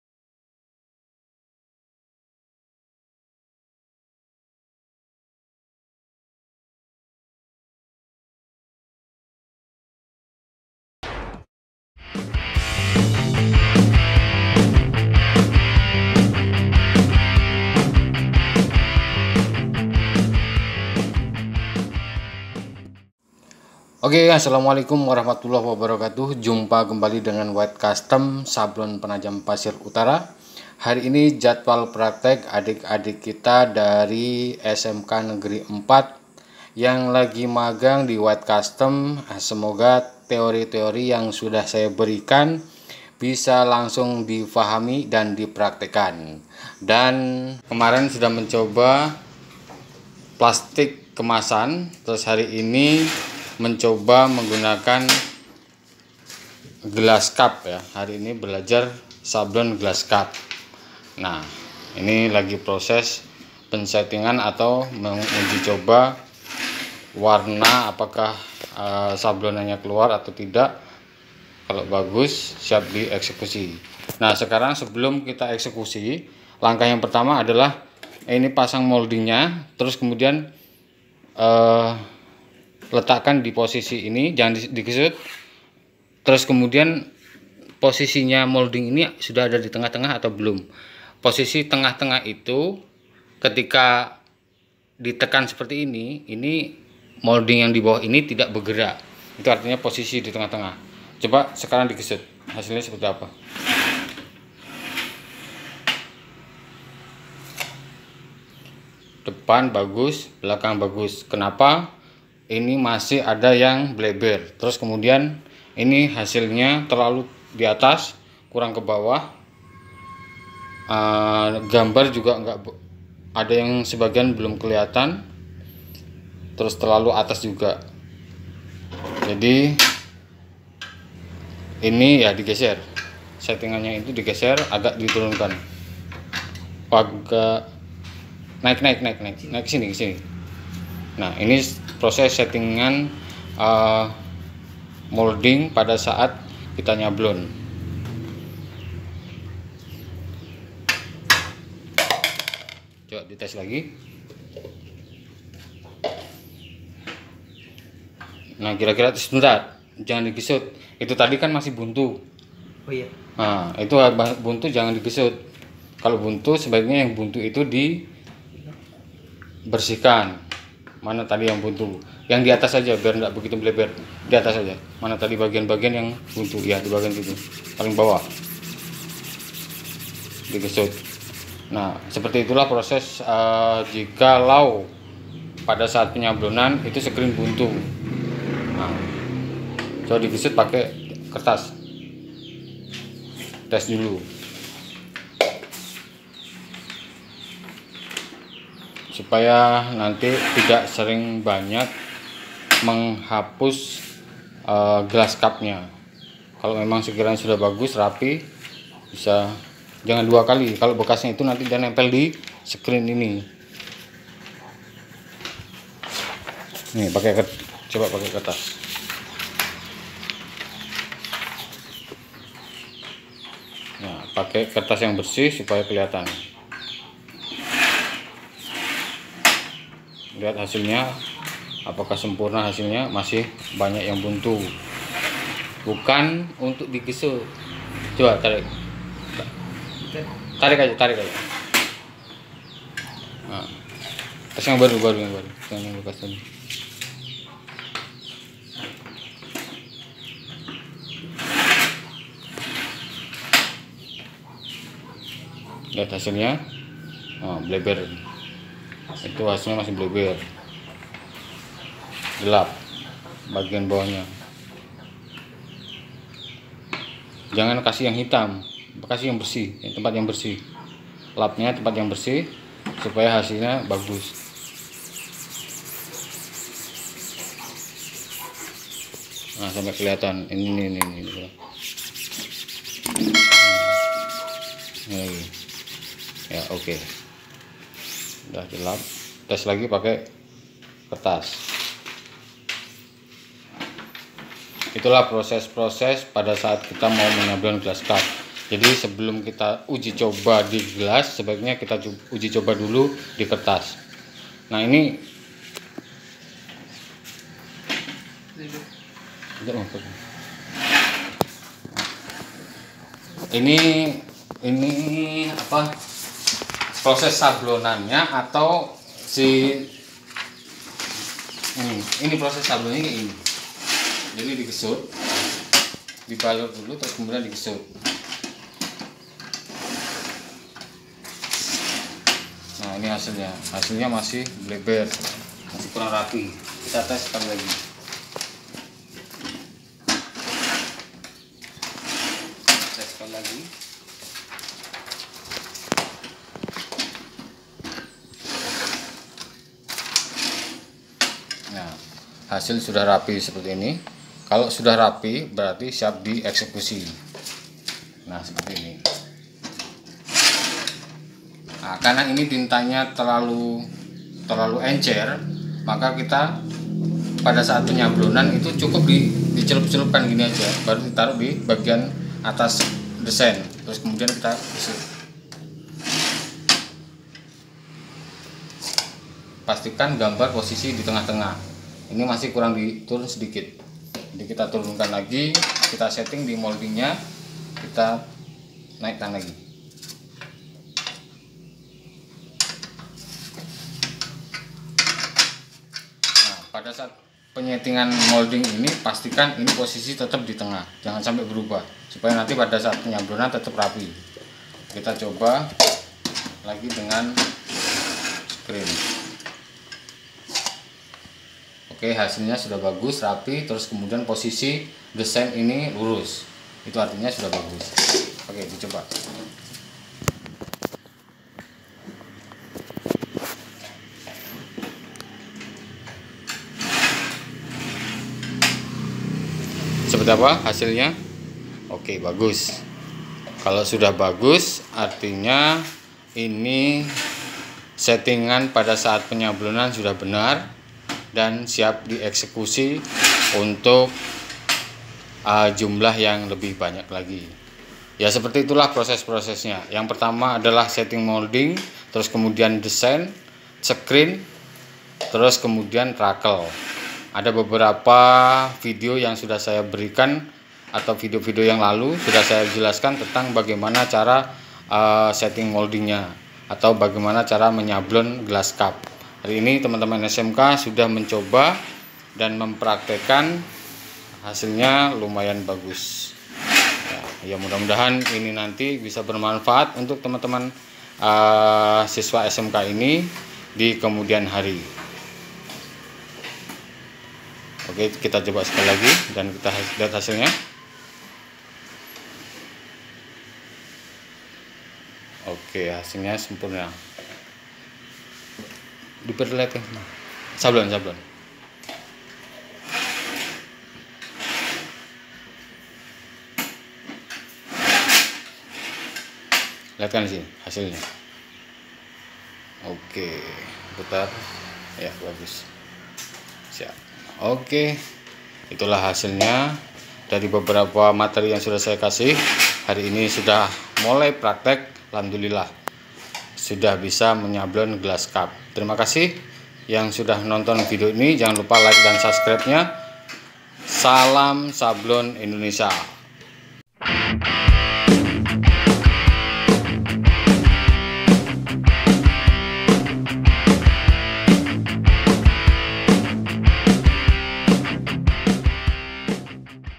Belajar SABLON GELAS CUP siswa PKL sampai BISA
Praktek sablon Gelas Cup siswa SMKN 4 Penajam Paser Utara yang lagi magang di Wide's Custom
Alat dan bahan yang diperlukan :
1. ALat sablon gelas cup Manual
2. Tinta plastik Polymate
3. Screen Modifikasi T180
4. M4 Super / Murni
5. Rackel Lancip
6. Gelas Cup 16oz